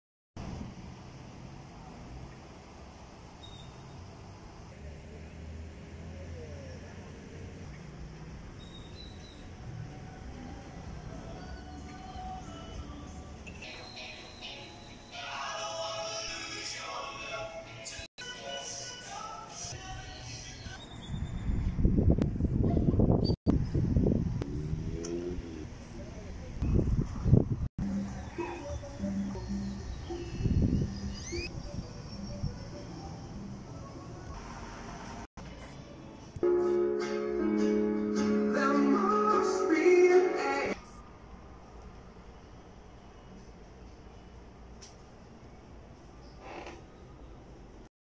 Benditas Redes Sociales Cancún
Bomberos Cancún trabajando con la seguridad usando cubreboca